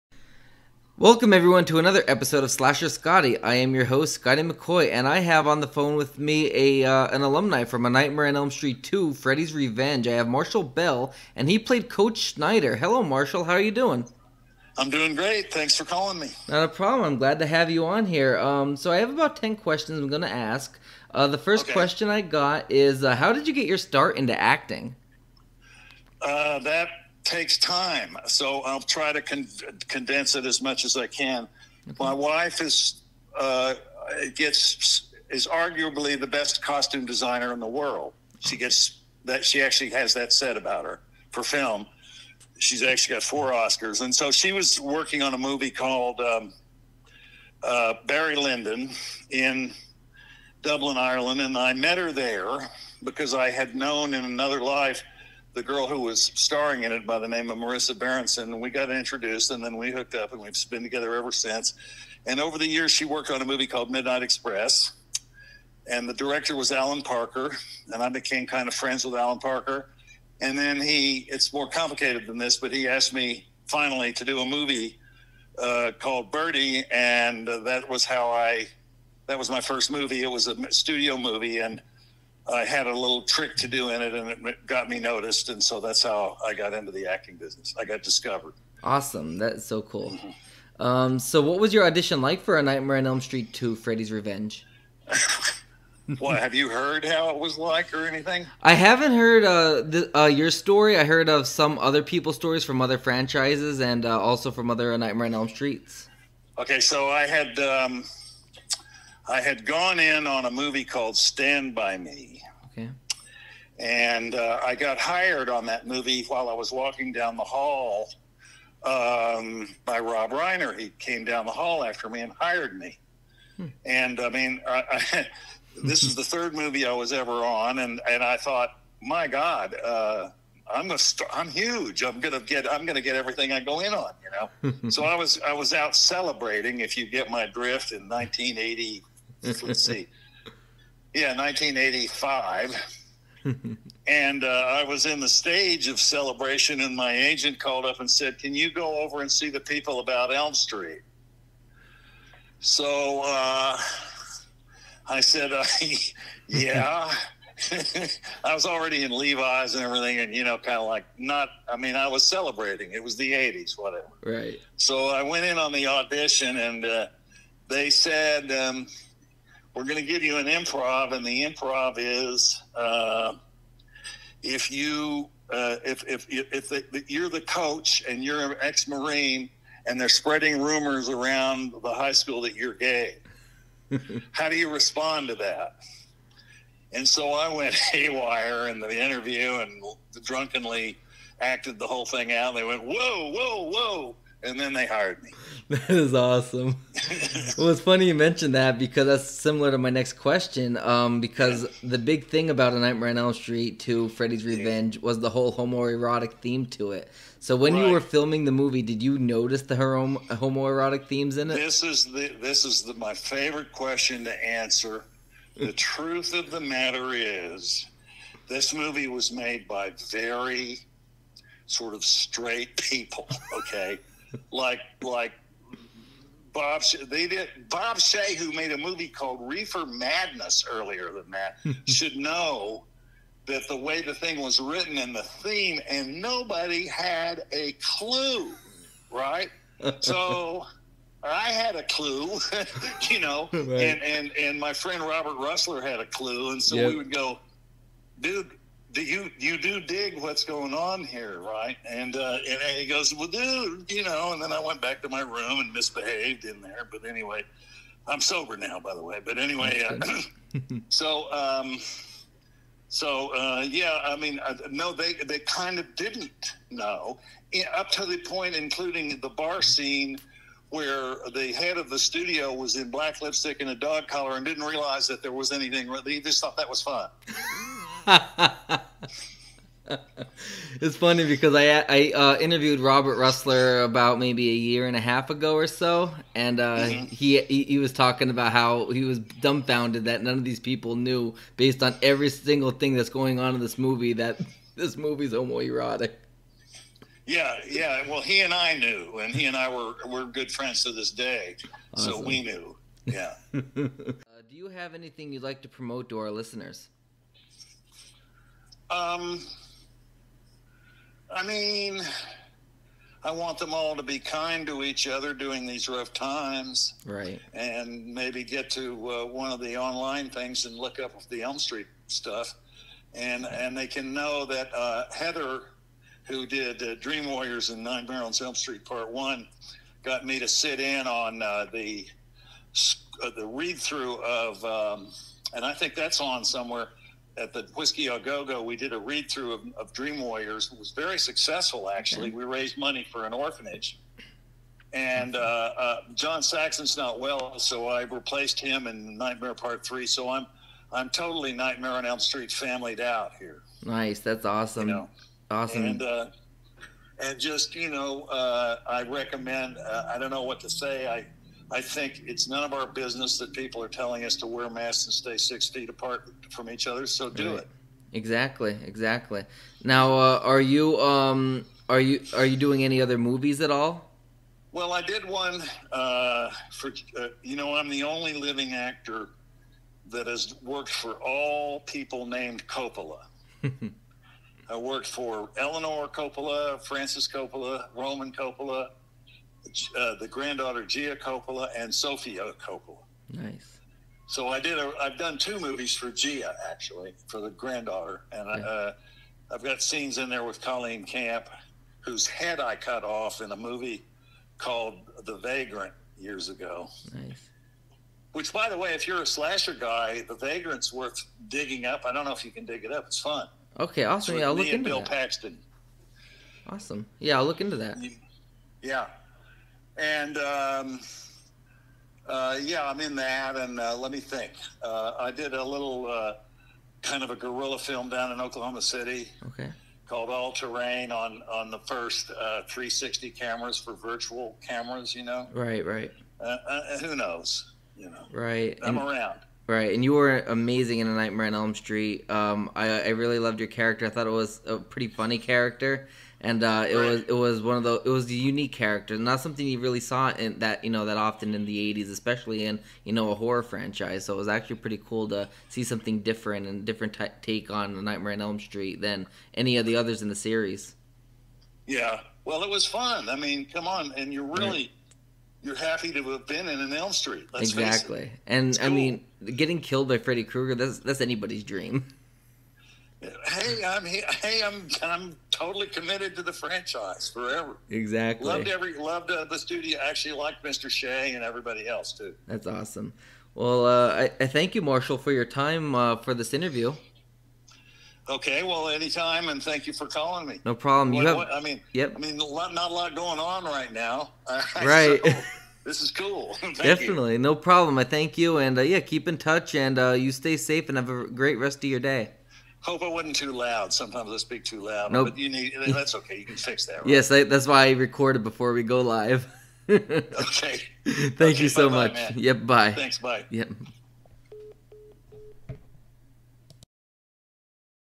Marshall Bell Interview Sneak Peek
The sneak peek of my interview with Marshall Bell, who played Coach Schneider in A Nightmare on Elm Street 2:  Freddy's Revenge.